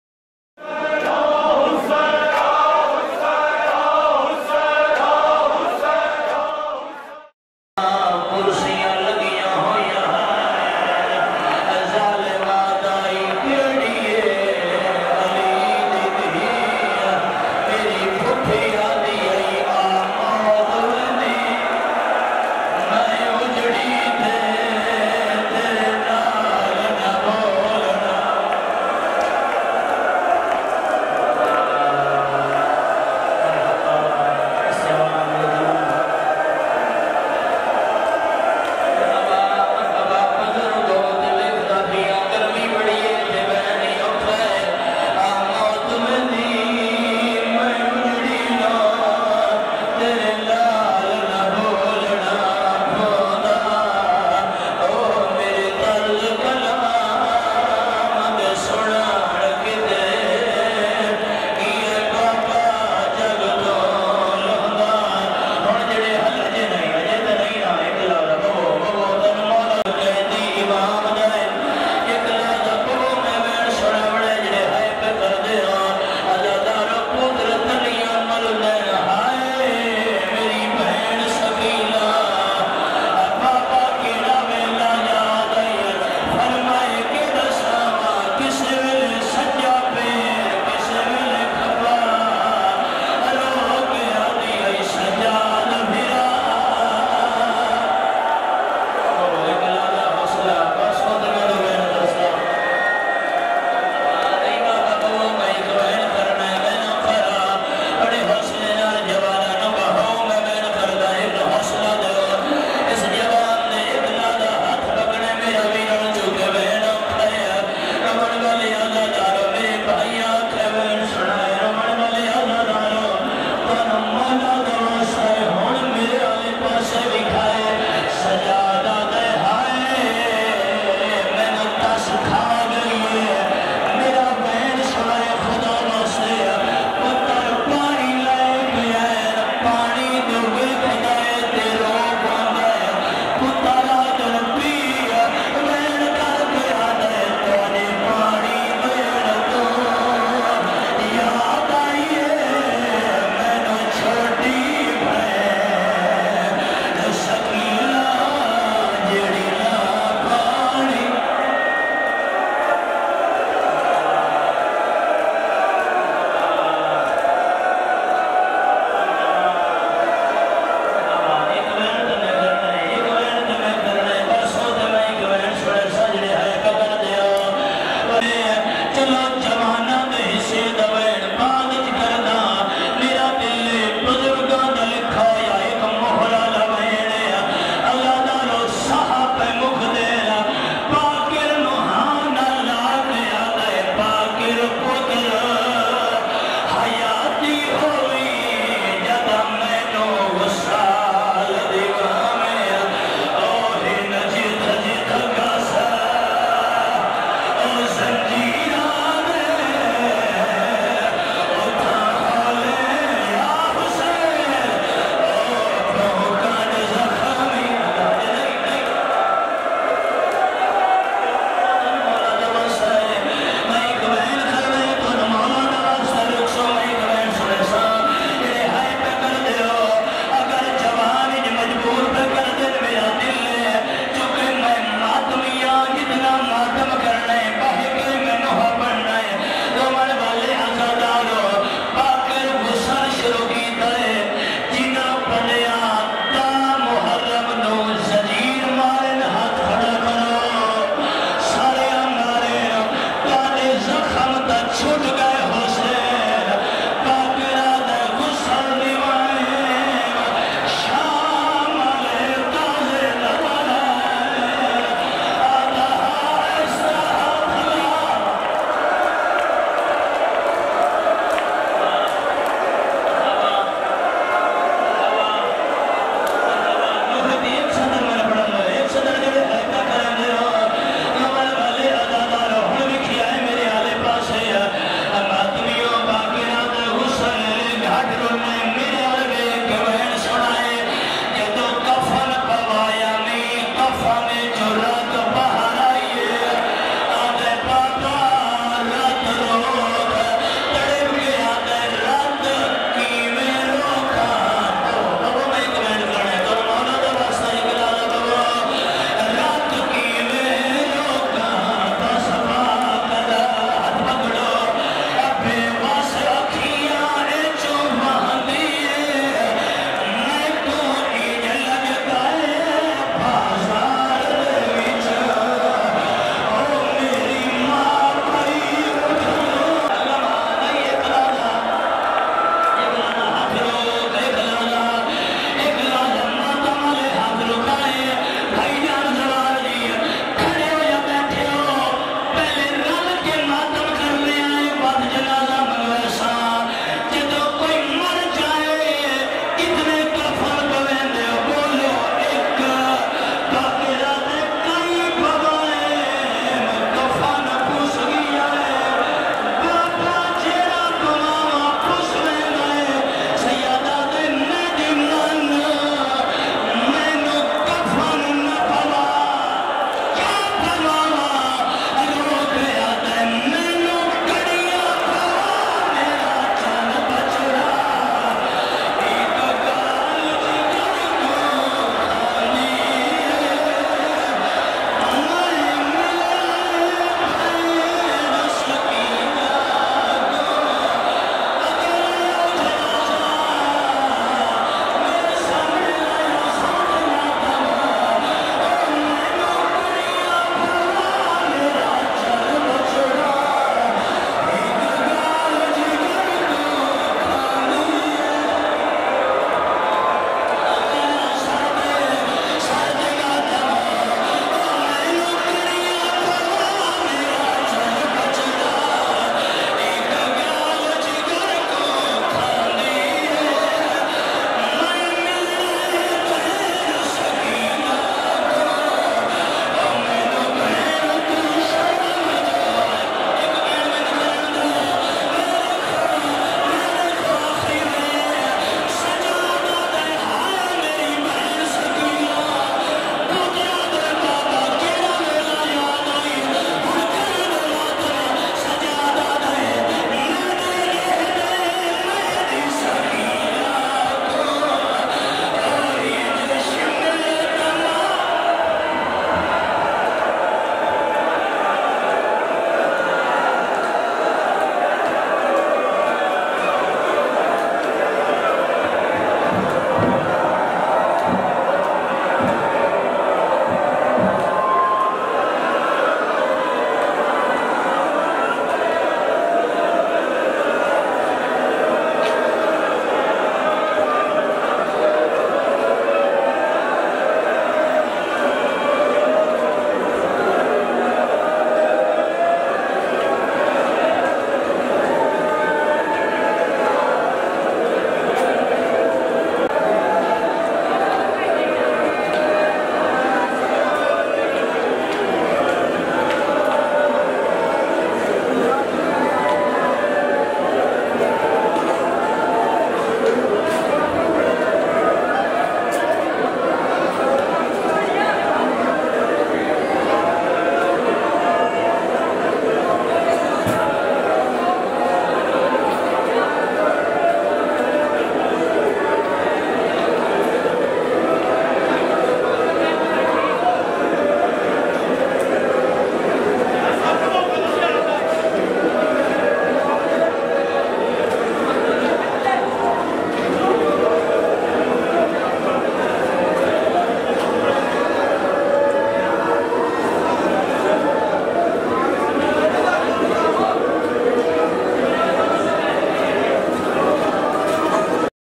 Zakir Aalam Abbas Bhatti I 25 Moharram 2022 I Baramdagi Taboot
سالانہ مجلسِ عزا بسلسلہ شہادت امام علی زین العابدینؑ
بمقام: مرکزی امام بارگاہ درِ بتولؑ اڈہ پسروریاں سیالکوٹ

خطاب و برآمدگی تابوت: ذاکر زوار حاجی عالم عباس بھٹی صاحب آف لیہ

Zakir Aalam Abbas Bhatti
On Dated: 25 Moharram 2022
Baramdagi Taboot Imam Sajjad (a.s)
@ Markazi Imam Bargah Dar-e-Batool (s.a) Adda Pasrorian Sialkot

Special Thanks for Video: SYED MESAM NAQVI (SJZ)
Duago: TEAM AZADARI IN SIALKOT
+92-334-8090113